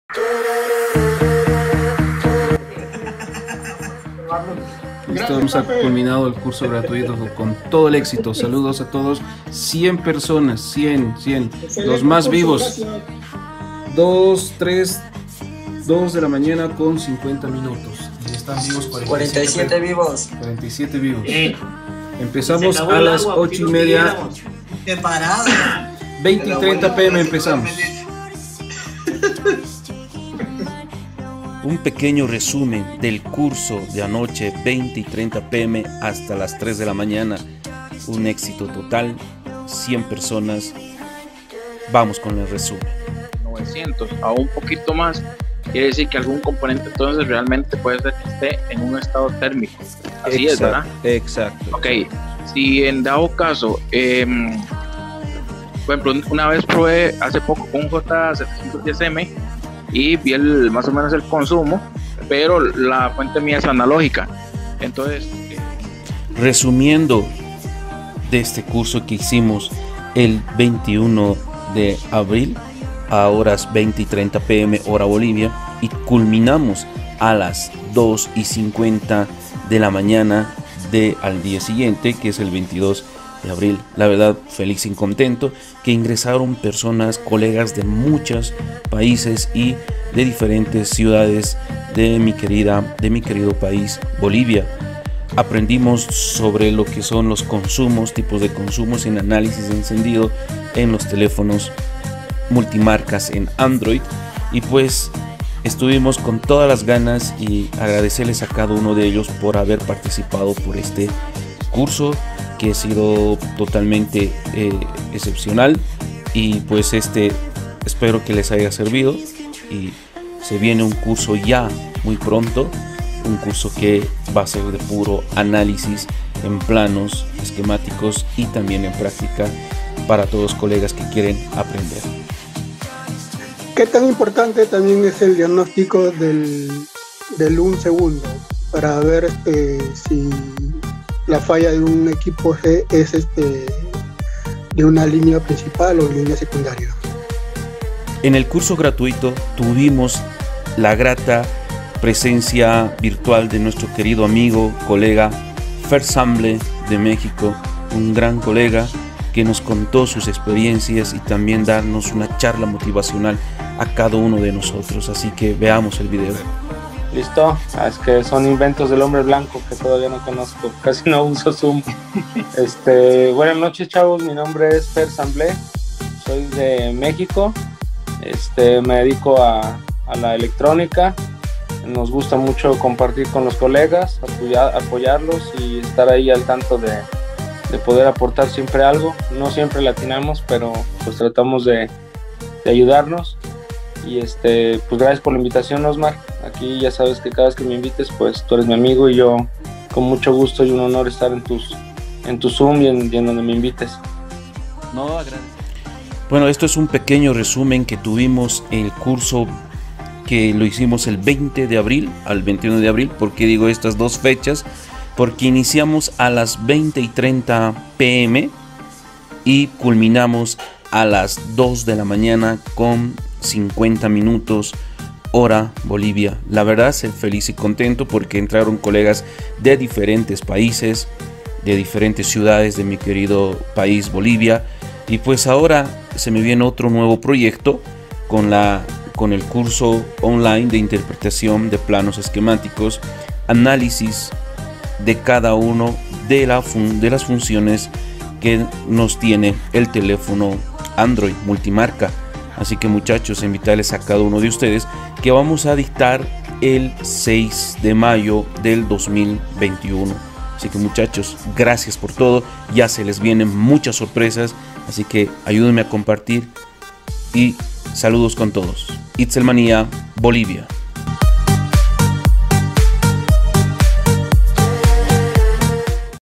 RESUMEN CURSO WEBINAR ANALISIS DE ENCENDIDO ANDROID. INVITADO ESPECIAL FER ZAMBLE (MEXICO)
hola queridos amigos, el dia miércoles 2 de abril tuvimos el grato de hacer un curso webinar, análisis de encendido android gratuito la verdad muchos colegas de diferentes paises y de mi pais Bolivia estuvieron presentes. saludos a todos.
no te olvides que daremos otro proyecto grande curso online análisis de fallas e interpretación de planos esquematicos.
cualquier consulta para curso online 6 mayo 2021 puedes ingresar a mi fan page itcelmania